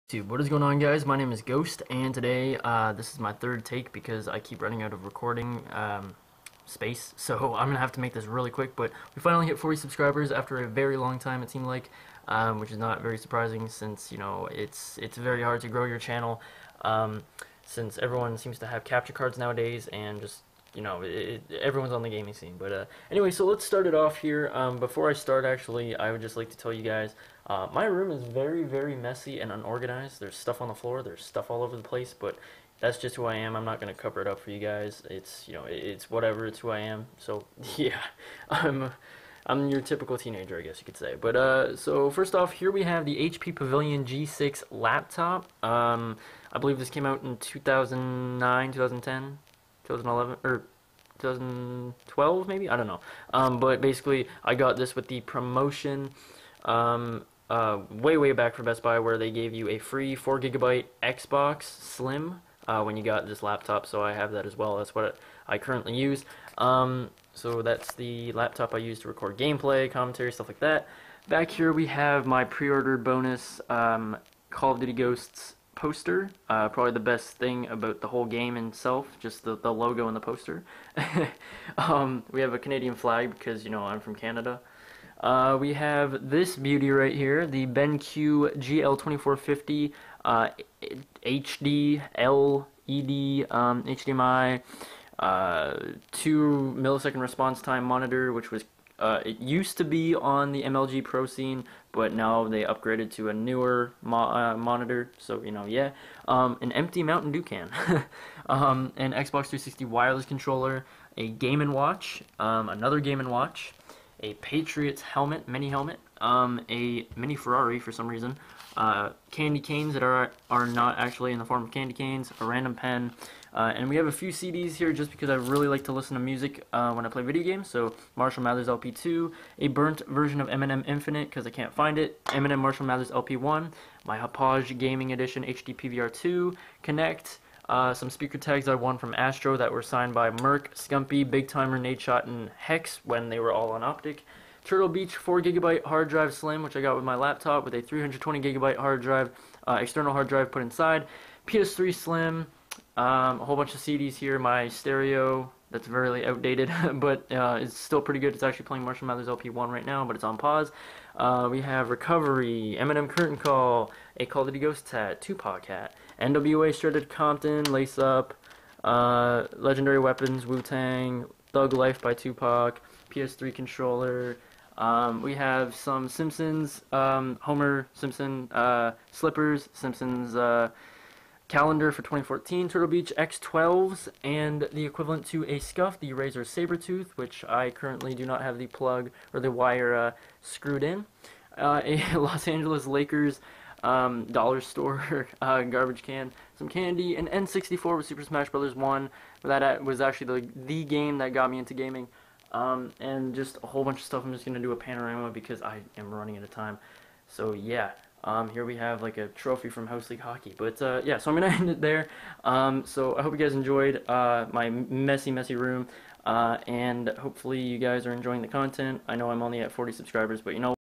Thanks for 40 Subs|Gaming Setup/quick Room Tour
Sorry for the unfortunate cut off at the end, I ran out of recording space, and didn't realize it. Hope you guys enjoyed, and thanks for 40 subs, can't wait until 50 then 100, and so on. Thanks again and as always guys it has been your boy G H O S T, and I hope to see you guys on my next video!